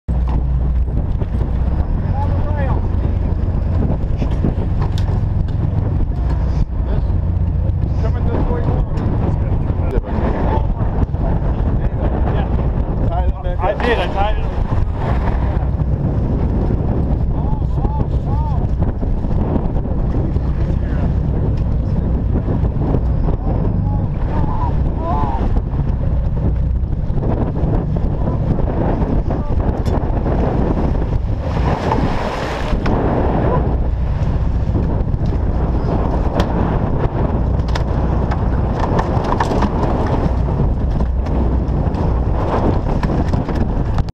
Hot, Rough Salmon Fishing on Blue Runner Sportfishing Charter
Video, courtesy of David Apilado Jr.